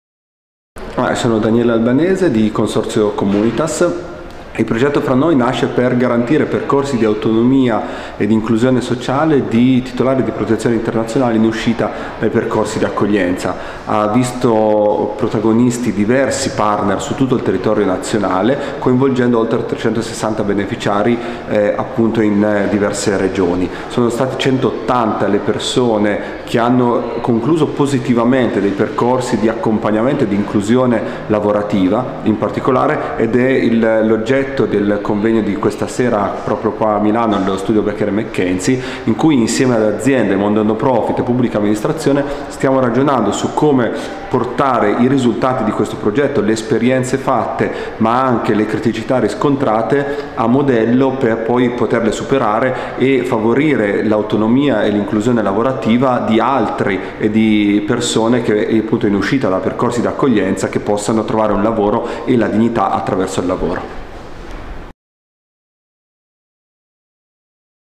Accompagnamento e inclusione lavorativa. Intervista a Daniele Albanese
In occasione dell'evento "L'impresa che include", che si è svolto il 7 novembre a Milano, Daniele Albanese, di Consorzio Communitas, racconta l'obiettivo del progetto Fra Noi, che ha garantito a 180 persone l'indipendenza lavorativa, grazie alla collaborazione con aziende, organizzazioni no profit e pubbliche amministrazioni.

Per saperne di più sul progetto